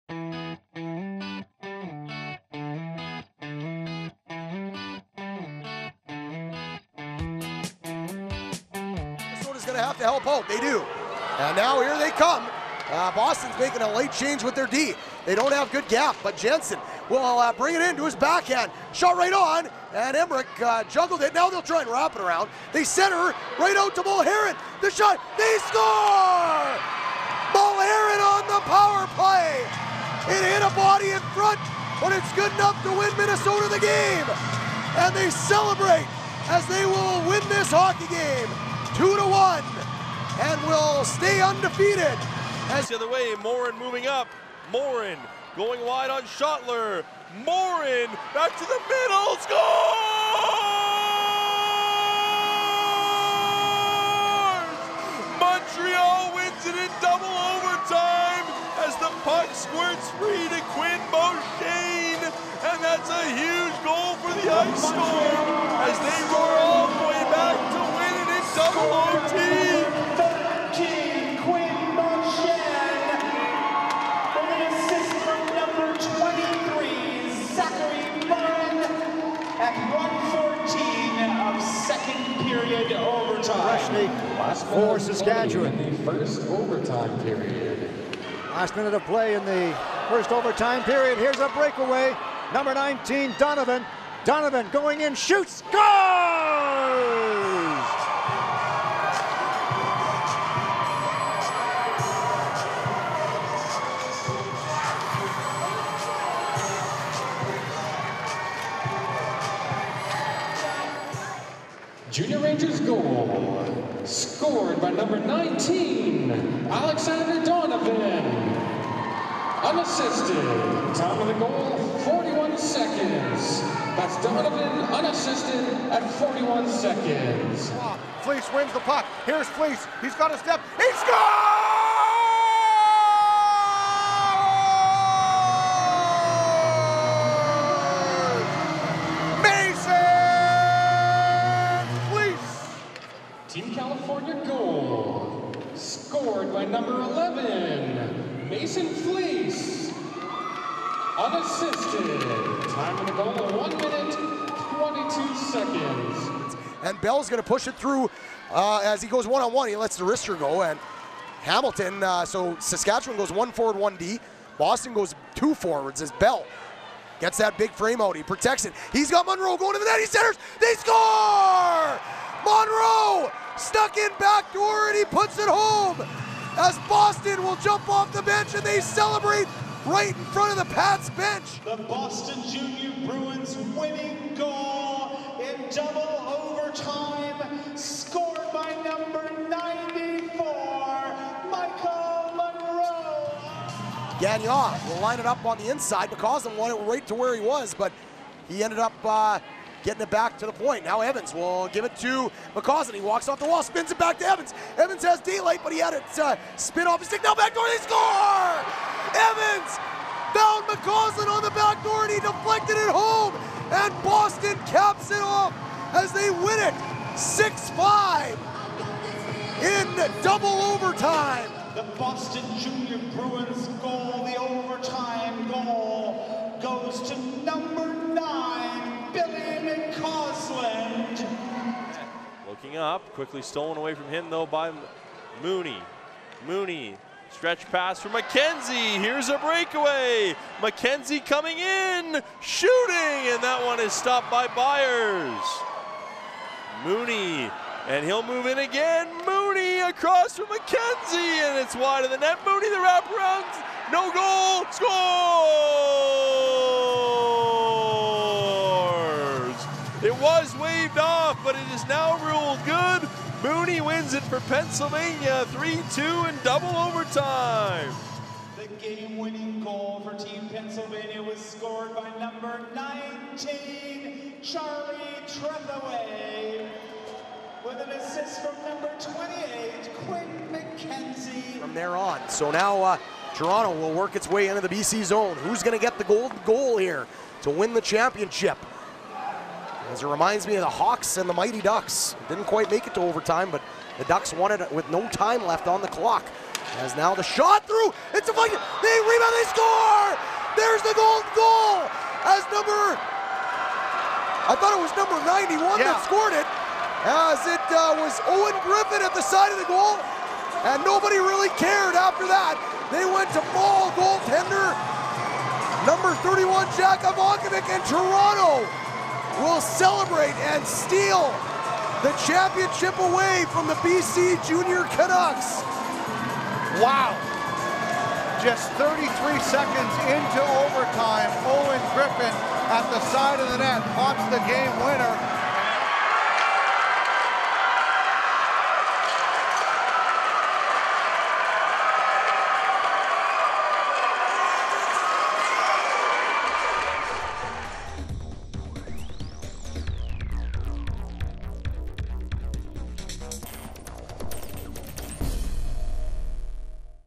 2017 - Overtime Magic - Brick Invitational Tournament
Brick Tournament Hockey Videos Goals and Saves from OT at the 2017 Brick Invitational Hockey Tournament at West Edmonton Mall in Edmonton, Alberta, Canada. This video is of the 2017 goals scored and saves made in overtime. Great players and great plays!

This video was filmed at the Brick Invitational Hockey Tournament at West Edmonton Mall in Edmonton, Alberta, Canada.  This video is about kids who are hockey superstars at the very young ages of 9 and 10.  It's incredible what they can do on the ice!